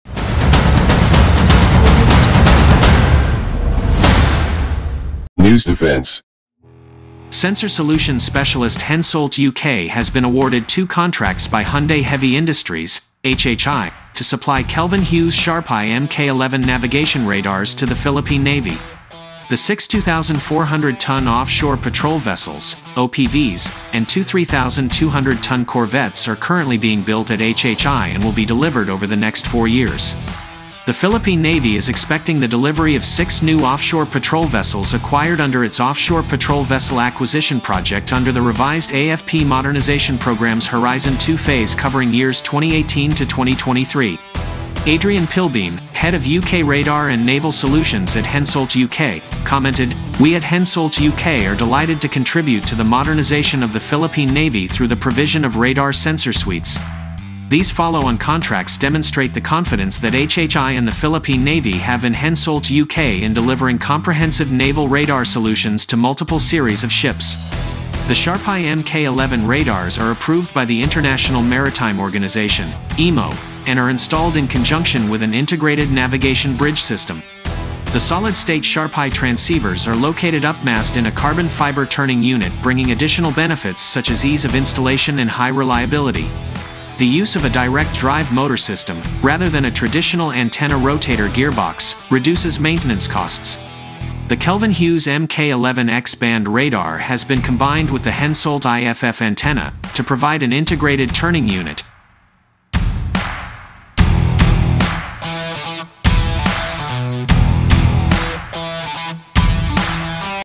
The Philippine Navy ordered the Kelvin Hughes SharpEye Mk11 navigation radar for new ovt
The SharpEye Mk11 radars are approved by the International Maritime Organization (IMO) and are installed in conjunction with an integrated navigation bridge system. The solid-state SharpEye transceivers are located upmast in a carbon fibre turning unit bringing additional benefits such as ease of installation and high reliability. The use of a direct drive motor system, rather than a traditional antenna rotator gearbox, reduces maintenance costs. The Kelvin Hughes Mk11 X band radar has been combined with the HENSOLDT IFF antenna, to provide an integrated turning unit, bringing SharpEye surveillance radar capability together with an IFF antenna system.